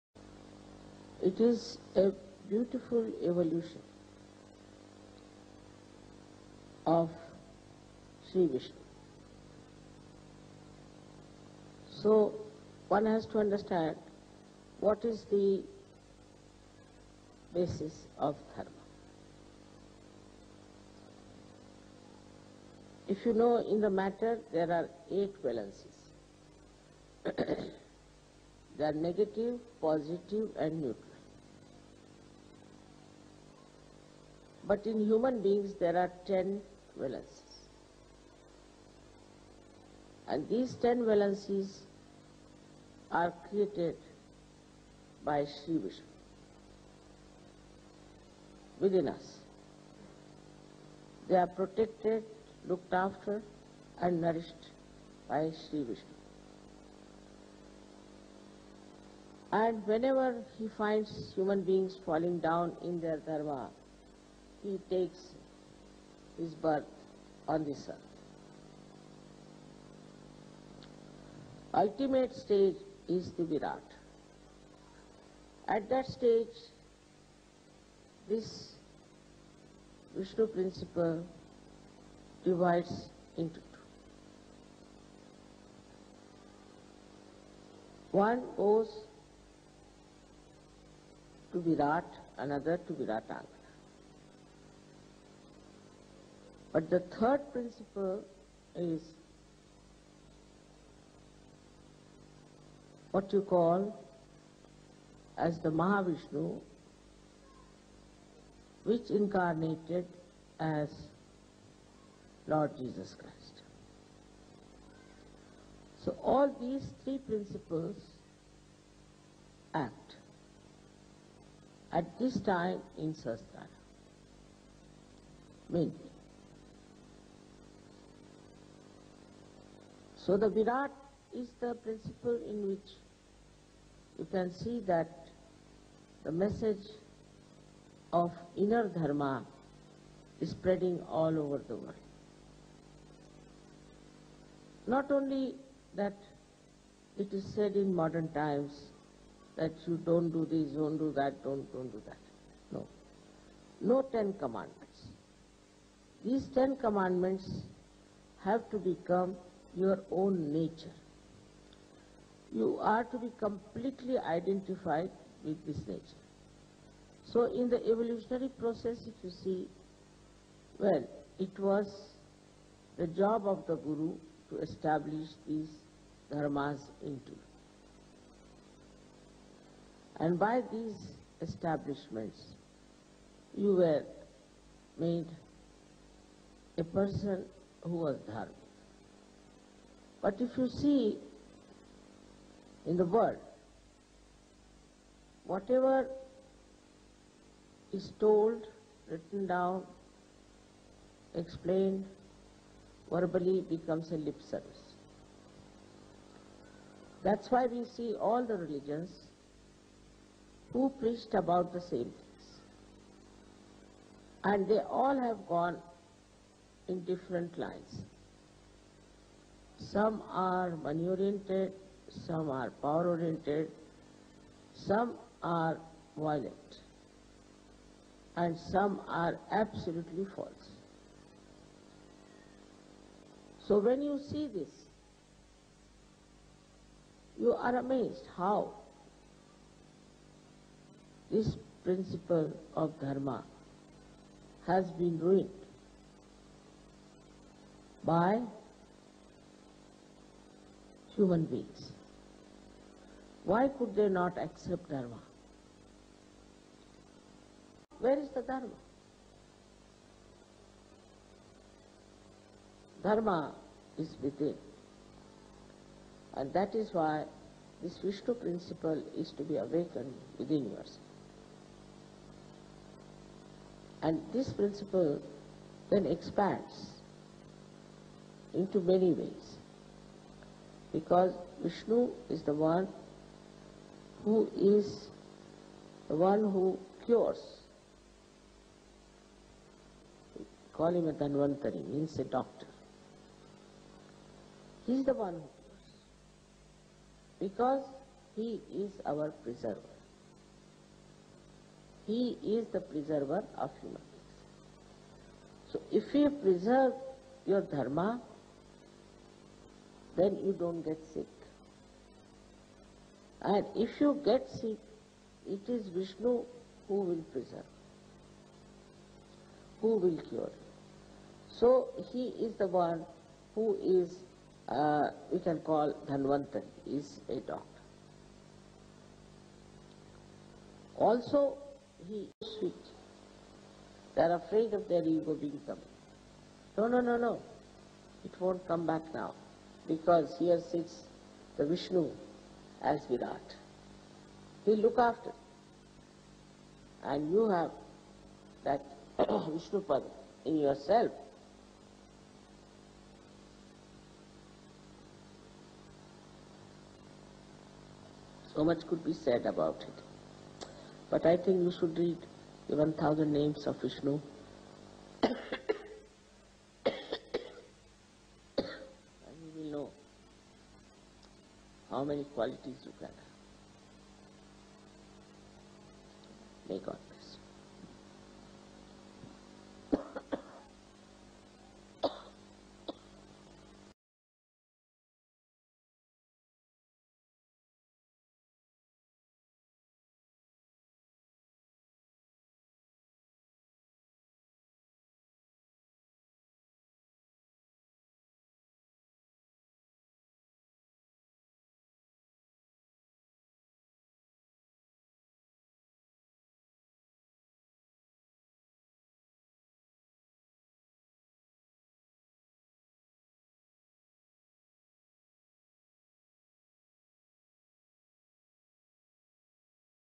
Shri Vishnu: dharma, religion & health
"So now, where is the dharma? Dharma is within, and that is why this Vishnu principle is to be awakened within yourself. And this principle then expands into many ways. Because Vishnu is the one who is, the one who cures. Wecall Him a Dhanvantari – means a doctor. He is the one who cures because He is our preserver; He is the preserver of human beings. So if He preserve your dharma, then you don’t get sick. And if you get sick, it is Vishnu who will preserve, who will cure you. So He is the one who is, we can call, Dhanvantari – is a doctor." Shri Vishnu puja , Paris, France 13th July 1994